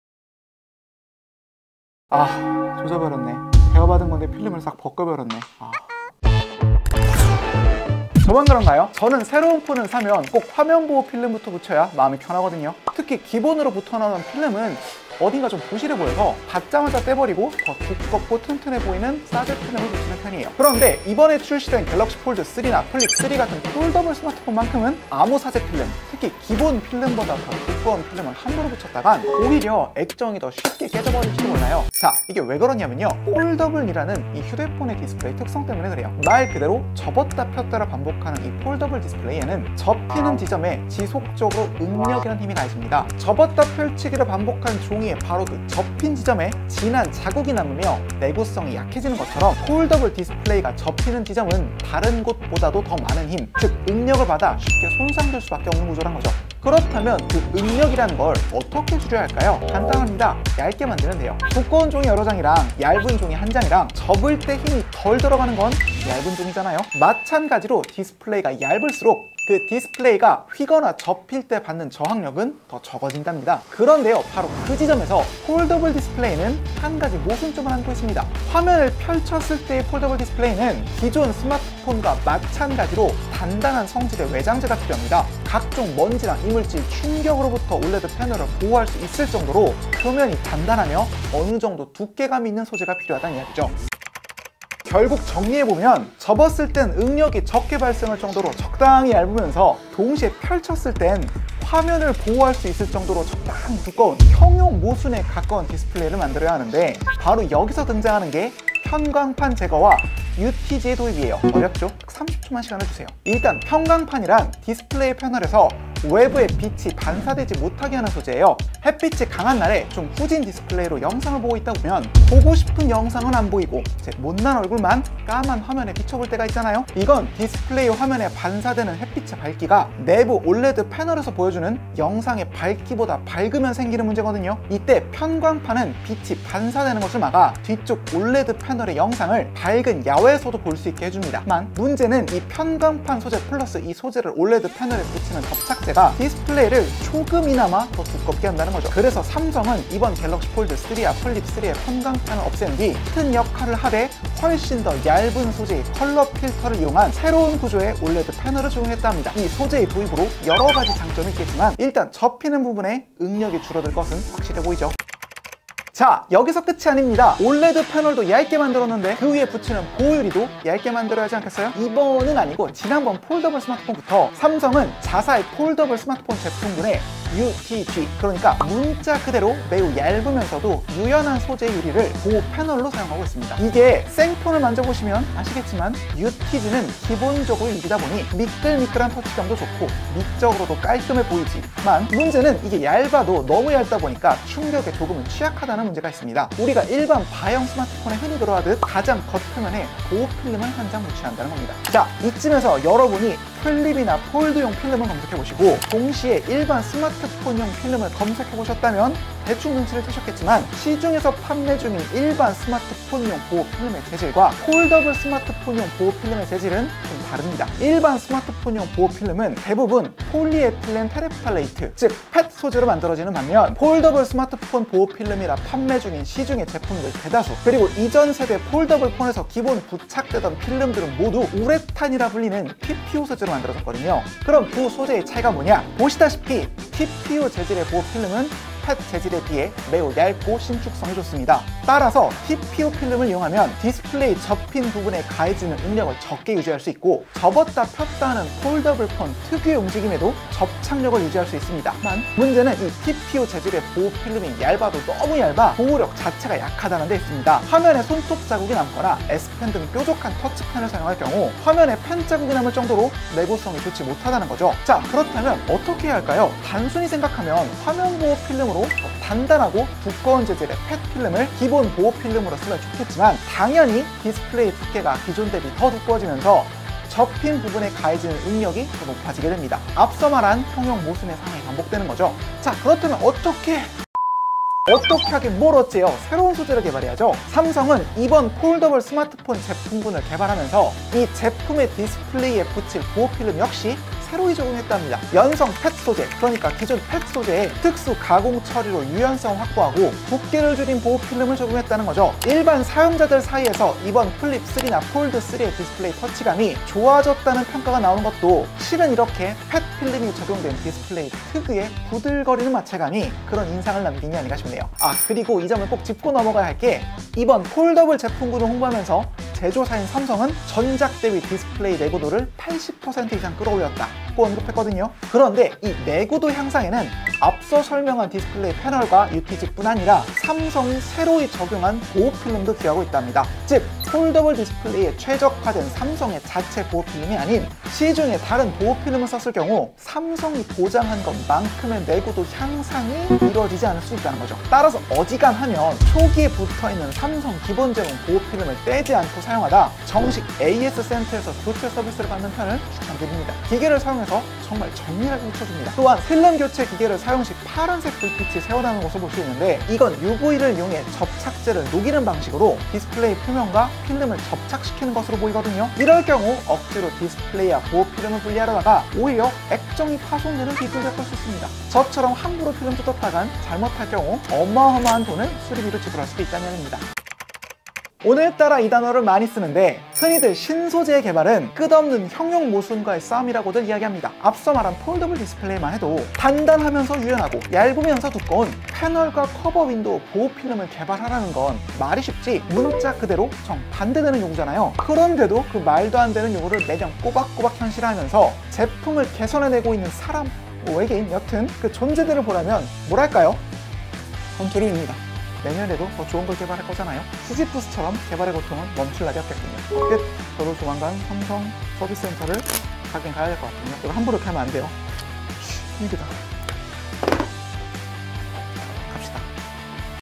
갤럭시 Z플립3 보호 필름, 함부로 떼면 큰일납니다 / [오목교 전자상가 EP. 53] 스브스뉴스
새로운 휴대폰을 사면 가장 먼저 할 일은? 소중한 신상 휴대폰에 '보호 필름'을 붙인다는 사람들도 많을텐데요, 최근 출시된 삼성의 폴더블 스마트폰에는 이런 행위가 오히려 '독'이 될 수 있다 합니다. 화면이 접히는 기기 특성 상 두꺼운 보호필름이 접히는 부위의 응력을 가중시켜 디스플레이가 파손될 수도 있다는 겁니다. 특히 응력은 접히는 부위가 얇을수록 그 힘이 적어지는 특징이 있어, 제조사인 삼성은 오랜 기간 자사의 기술력을 총 동원해 디스플레이의 두께를 줄이는 작업을 계속해왔다고 합니다. 디스플레이 구조를 최적화하고, 얇고 유연한 형태의 UTG 소재를 개발해 접힌 부위의 응력을 줄이는 작업을 반복해왔죠. 그런데 이렇게 두께를 줄일수록 펼쳤을 때의 디스플레이 내구도는 오히려 감소할 수 있다는 문제점이 있습니다. 얇은 유리 소재만으로 모든 충격을 방어하기엔 역부족일테니까요. 그래서 삼성은 이 디스플레이를 보호할 '보호 필름' 역시 새로 개발해 적용시켰다고 합니다. 접혔을 때 응력을 가중시키지 않을 만큼 얇고 유연하면서, 펼쳤을 때 각종 오염 물질로부터 디스플레이를 보호할 수 있을 정도로 두껍고 튼튼한 보호필름. 삼성은 대체 어떻게 이런 보호필름을 개발할 수 있었을까요? 지금 바로 영상으로 확인해보세요!